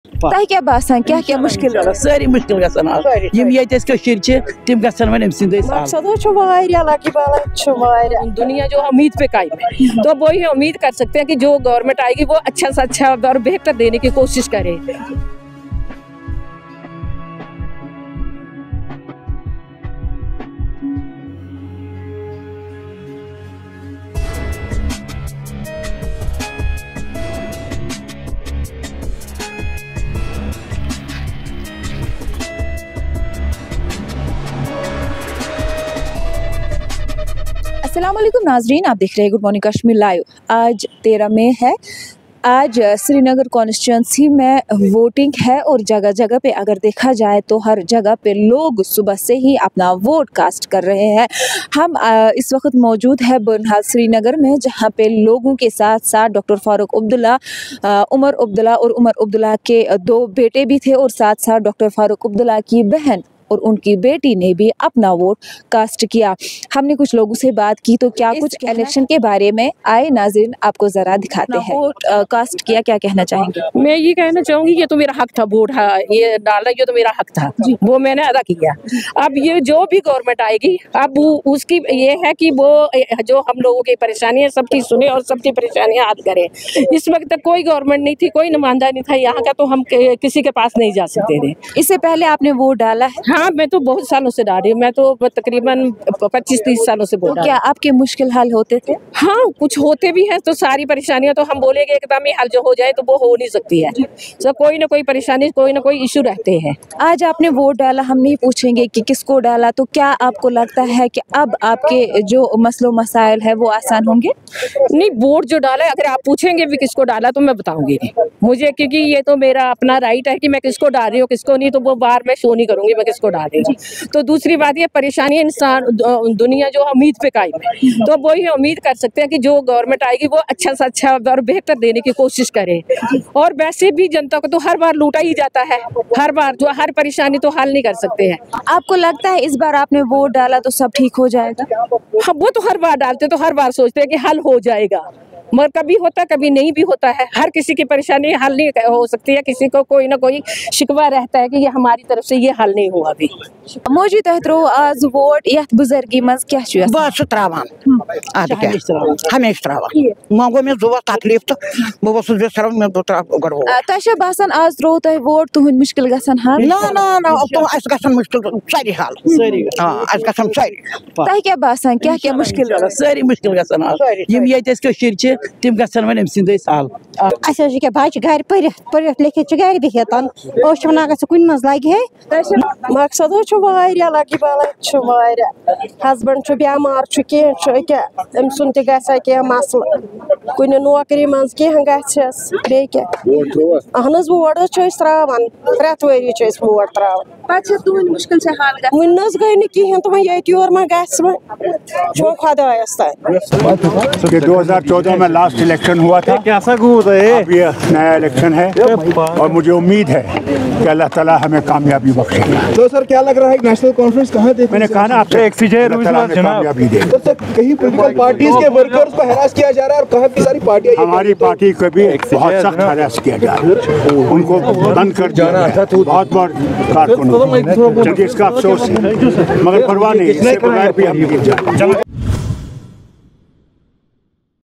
LS Polls: Voters turnout in large numbers in Srinagar
LS Polls ; Ground Report From BurnHall , Srinagar

Camera & Editing : Sartaj Altaf